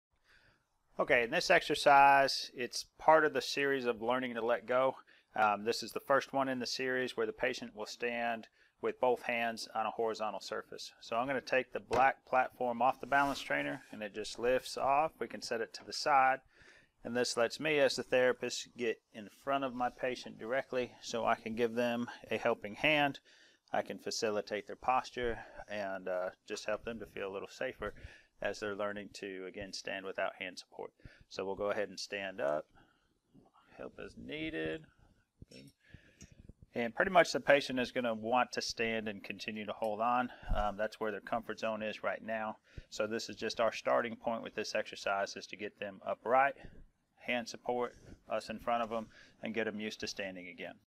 Exercise 34: Standing + Horizontal Hand Support
Start: Same as Static Standing, except patient holds onto horizontal supports for balance. Instructions: While holding horizontal supports, patient is directed to stand and try to minimize postural and/or balance deviations, e.g., equal weight distribution left and right and forward and back. Patient should not be leaning to one side, nor feeling heavy weight through heels or toes. Finish: At end of predetermined time period or stopped early if patient is unable to remain steady at least 20% of exercise time. Benefit: This is a lower-level exercise for patients learning to stand again. Therapist is typically positioned near patient to help spot and facilitate posture and balance as needed.